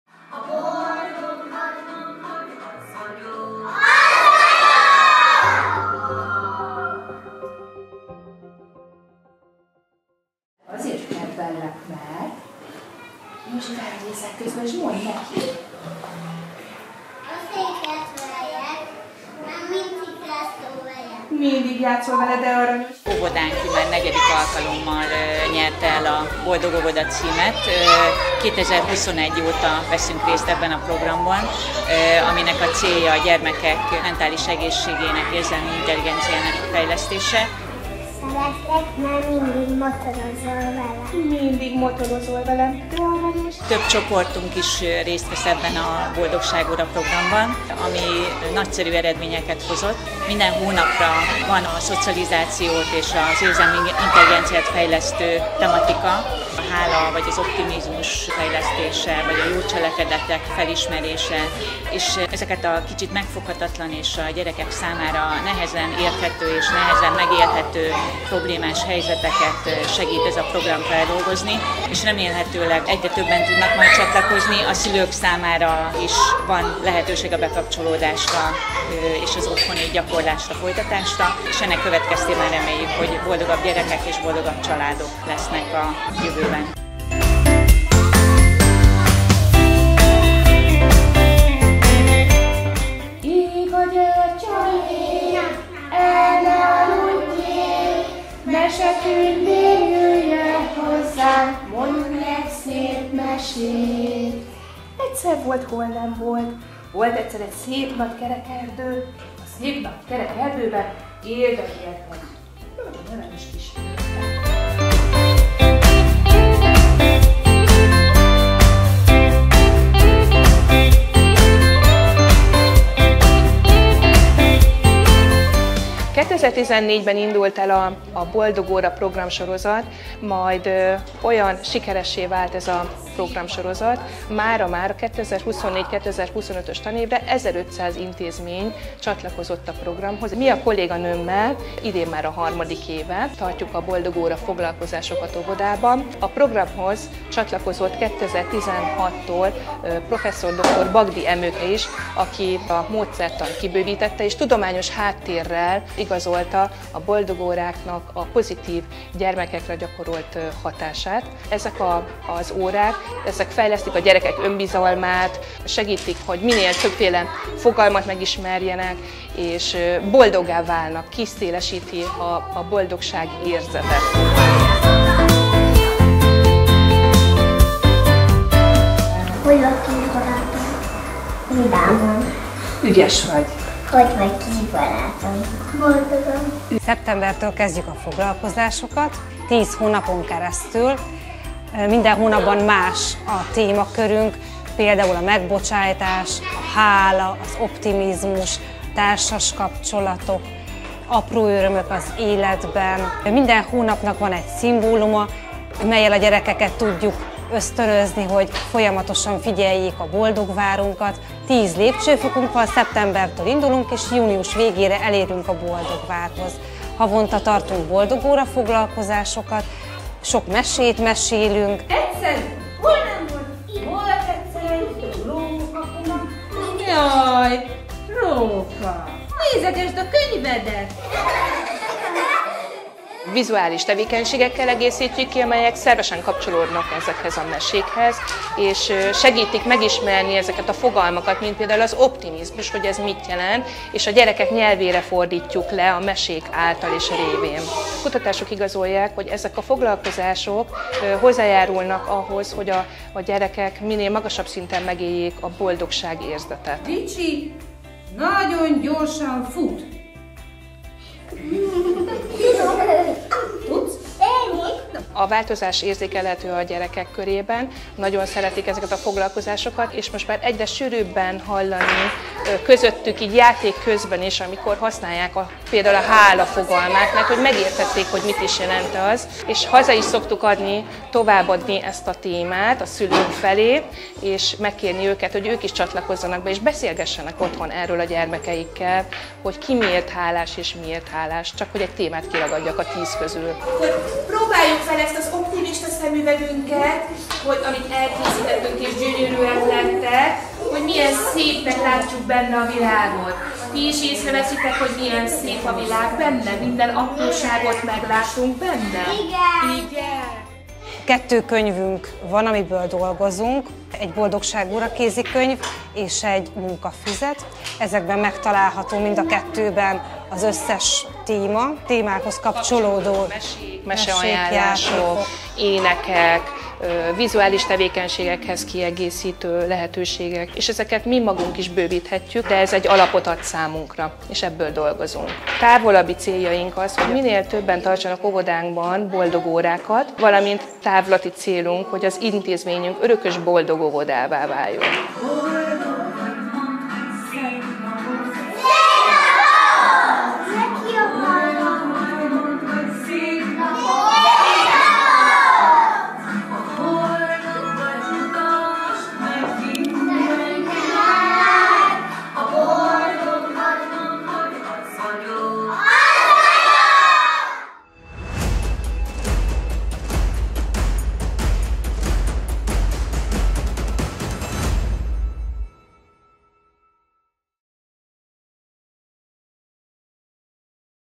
KISKUNMAJSA: NEGYEDIK ÉVE "BOLDOG ÓVODA" A KISKUNMAJSAI NAPSUGÁR ÓVODA
Október 25-én, Budapesten került átadásra egy Címátadó Nagyrendezvény keretében sok-sok intézmény számára a BOLDOG ÓVODA címet elismerő oklevél, amelynek a Kiskunmajsai Napsugár Óvoda is büszke és boldog tulajdonosa immáron 4. éve.